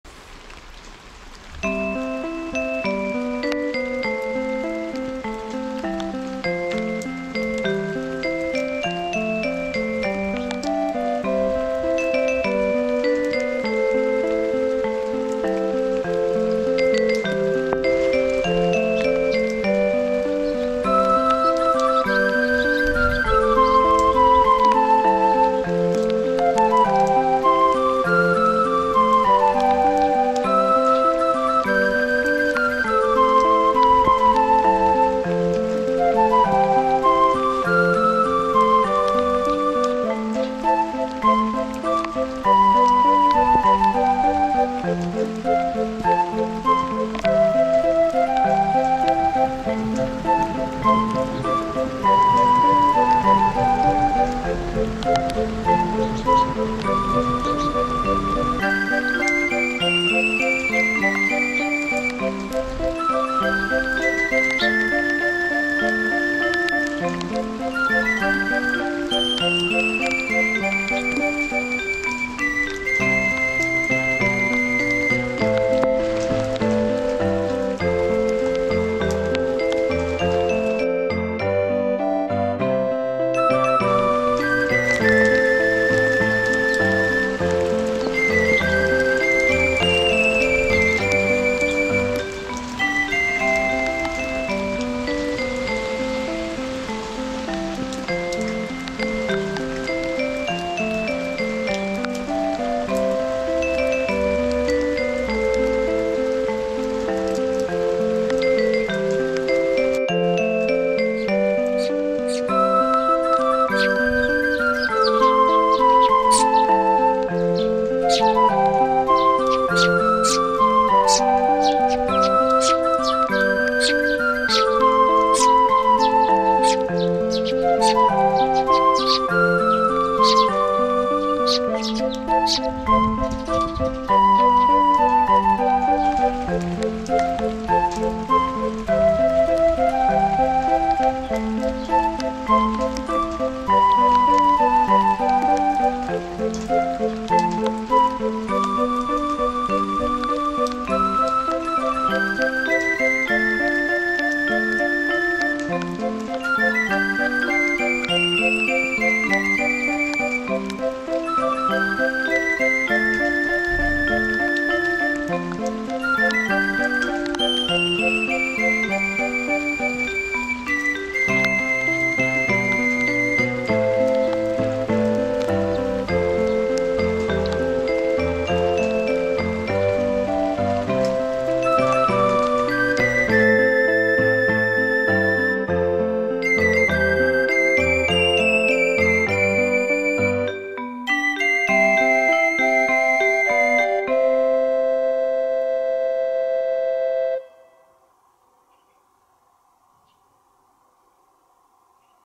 ずぶ濡れで遊ぶスズメのヒナと庭の花 手造り野鳥園 4K Sparrow chicks and garden flowers in the rain.
我が家の瓦屋根の隙間で生まれたスズメのヒナたちは雨が降っても楽しそうに遊んでいます。庭のスズメ観察。

Sparrow chicks born in the gaps in the tiled roof of our house are playing in the rain.

BGM 「音楽工房夢見月」さまにお借りしました。